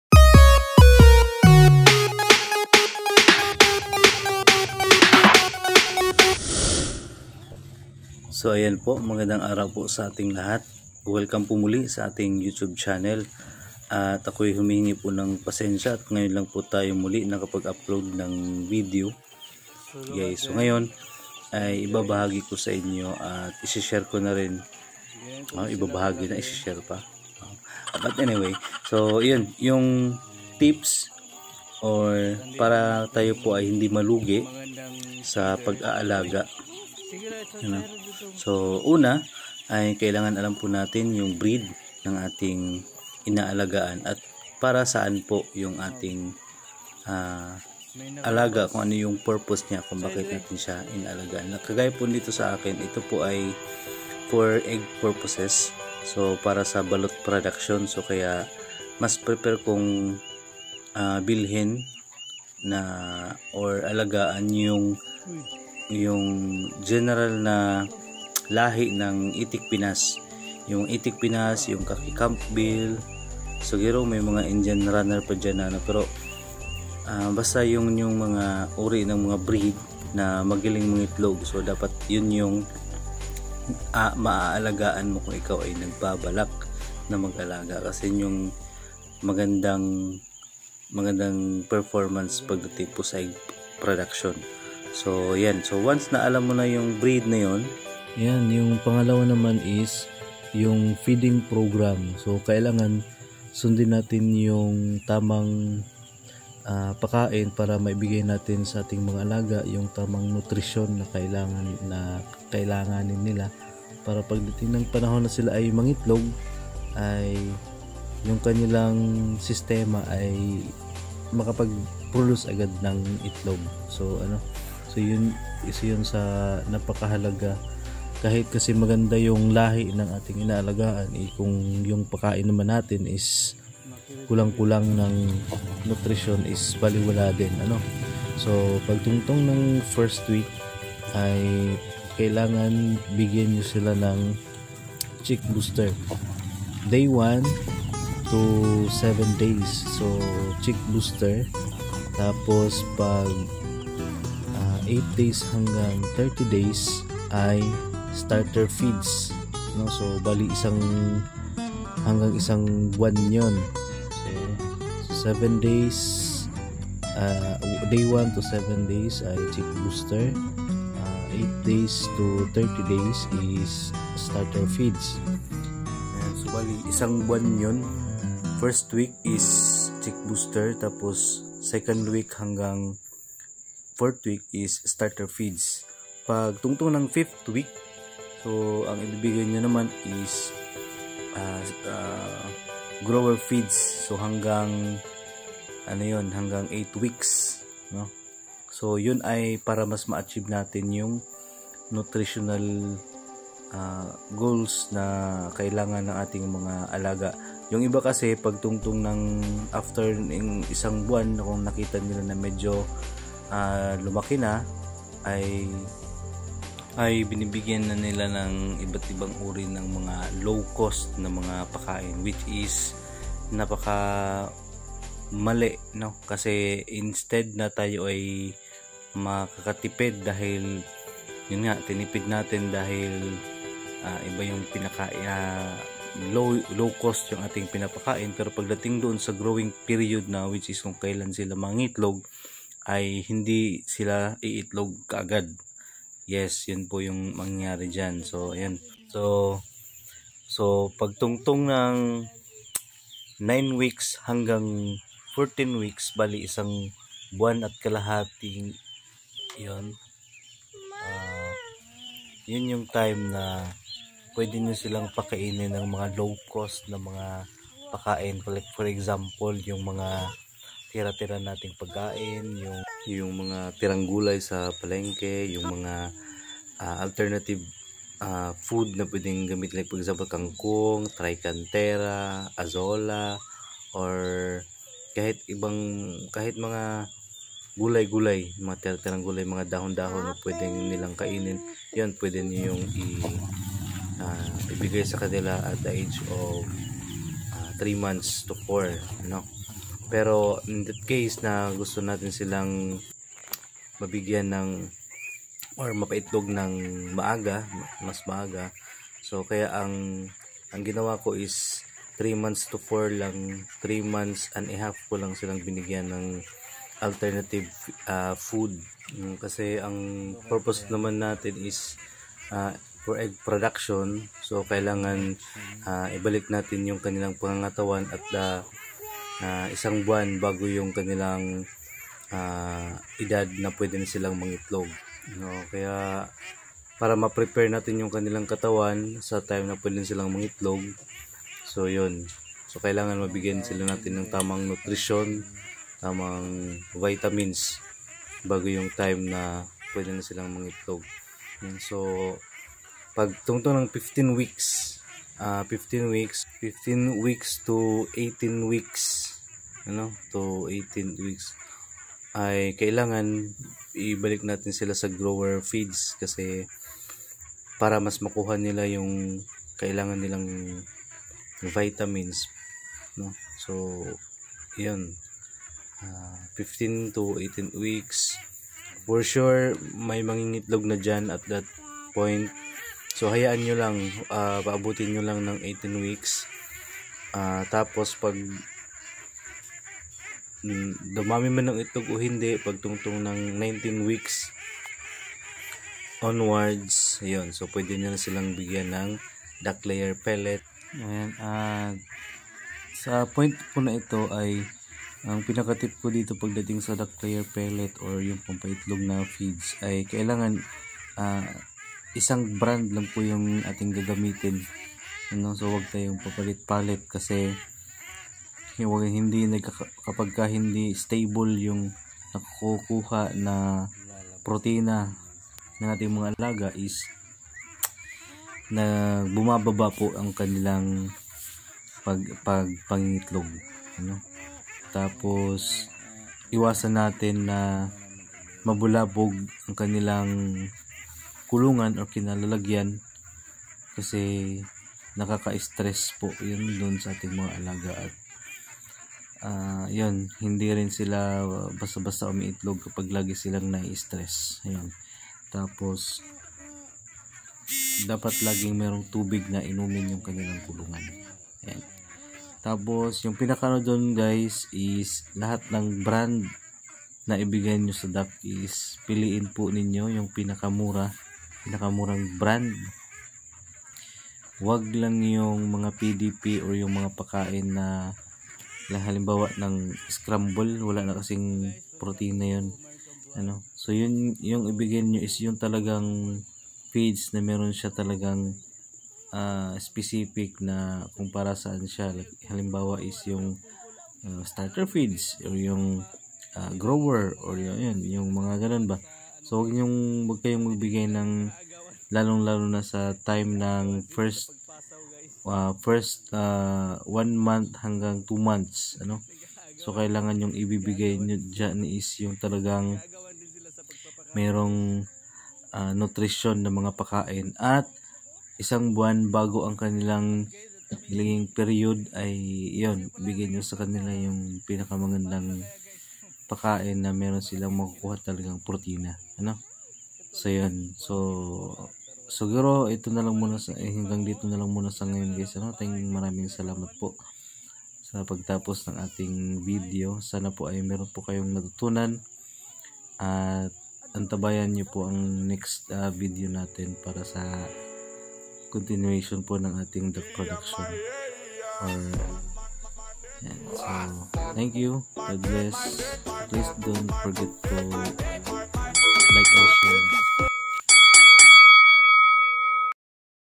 duck raising tipid tips program from chick to laying period.
tipid tips para hindi ma lugi sa duck raising 100% confinement system

watch tell the end inable for you to learn.
this program is base on my personal experience.

Week 1 -check booster
Week 2  - starter feeds
Week 3 - starter feeds
Week 4 - starter feeds
Week 5 -grower feess
Week 6 - grower feeds
Week 7- grower feeds
Week 8- grower feeds
Week 9 to week 14 - optional pwede nyo na sila bigyan ng mga low cost na mga pakain gaya ng darak, BO o mga rejected na feeds sa mga agrivit supply's, mga tirang gulay o rejck na gulay sa palingke, trikantera, kangkong, azolla, at ibapa as long as na kukuha ng mga itik ang quantity na kailangan nila sa pang araw araw.
Sa point na ito mag ka cost cutting po tayo sa pakain na hindi na lalagay sa alanganin ang kalusugan o growing period ng ating mga alaga.
Week 15 to 18 - grower feeds para ma ehanda ang kanilang systema sa laying stage.
Week 19 onwards - laying feeds.  Sa feeds ay pwede po ang pang baboy o pang manok na brand at piliin nyo ang pinaka mura na brand.
Ugaliin nyo rin na mag bigay ng vitamins once to twice a week.

Happy farming mga ka marcom tv

Leave your question sa comment section.
Salamat po